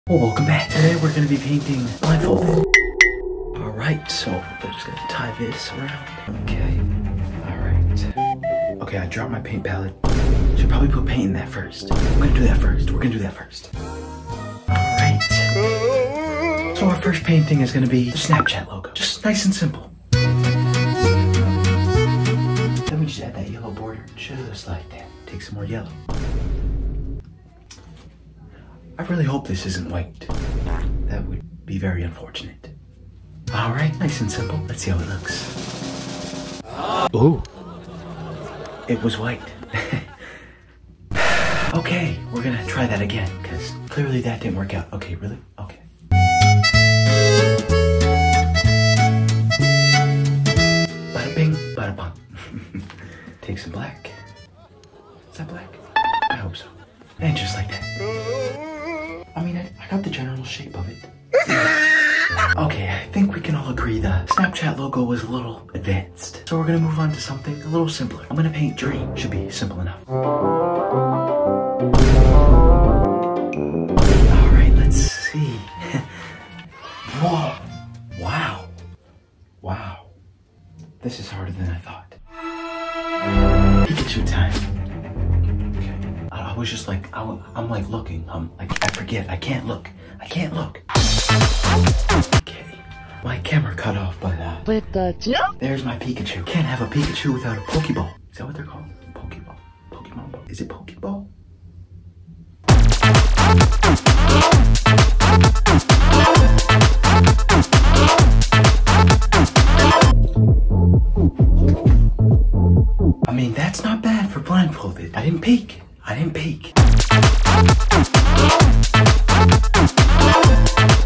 I Tried Painting While Blindfolded...
Hardest art challenge ever! Today I tried to paint blindfolded and It was so fun.

Today I didn't do my normal TikTok art / Influencer-celebrity portrait, I did something way better in my opinion. Painting blindfolded is harder than I though but was actually a fun challenge, I hope you enjoy!